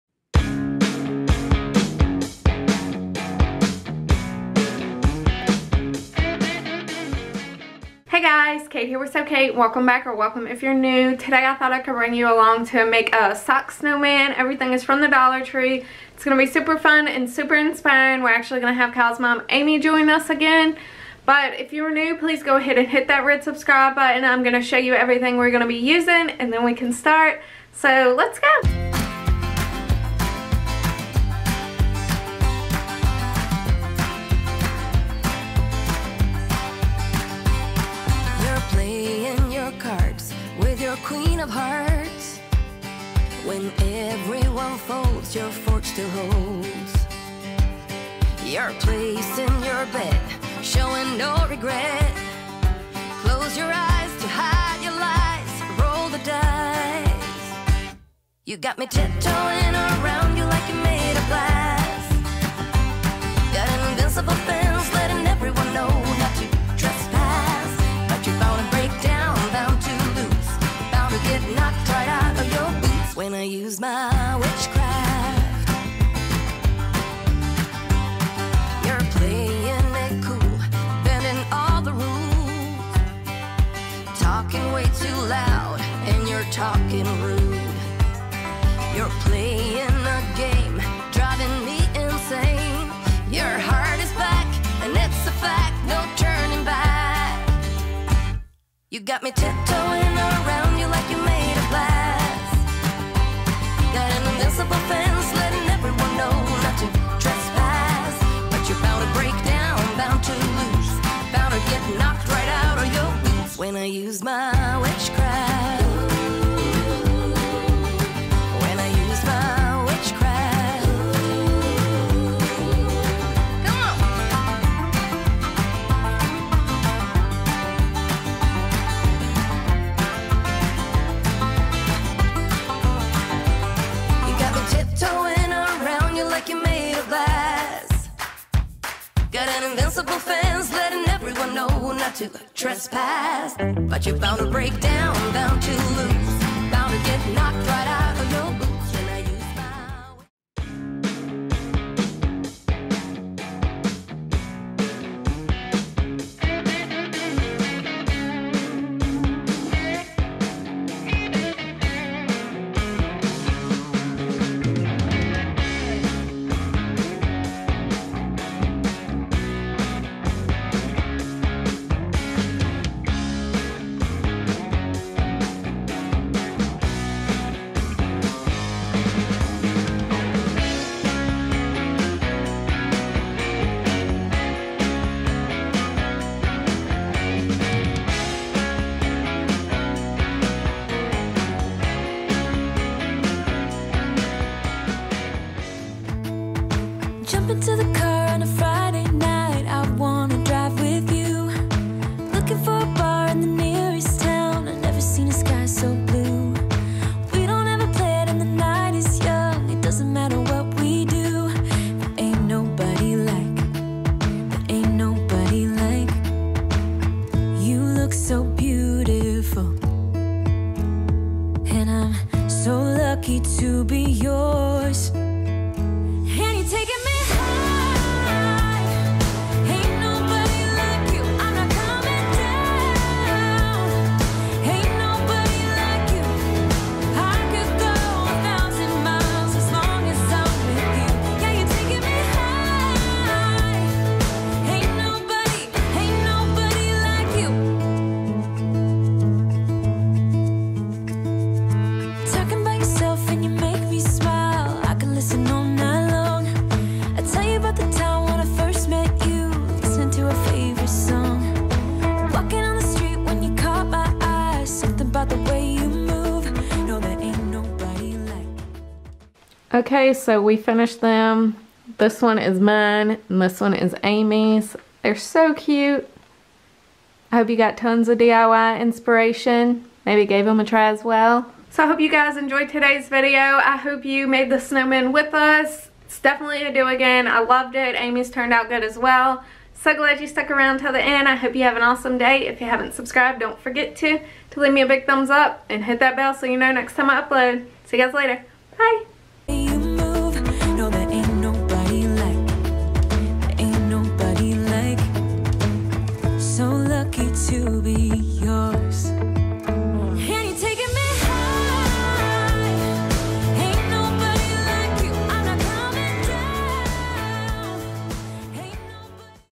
Dollar Tree DIY sock snowmen
Come along with me and Kyle's mom, Amy, for a fun Dollar Tree DIY sock snowman. We're going to be making snowmen using socks and rice. Hoping I can inspire you to try it.